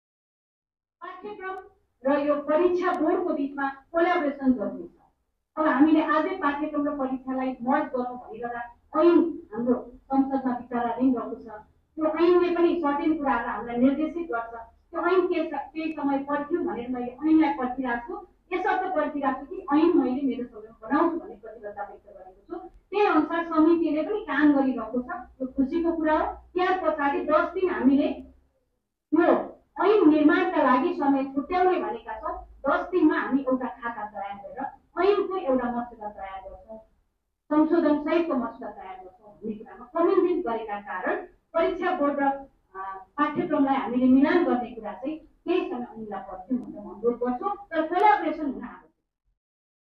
तिहारपछि १० दिन विद्यालय शिक्षा ऐन निर्माणका लागि समय छुट्याएका छौँ : शिक्षा मन्त्री भट्टराई, ।
तिहारपछि १० दिन विद्यालय शिक्षा ऐन निर्माणका लागि समय छुट्याएका छौँ : विद्या भट्टराई, शिक्षा मन्त्री ।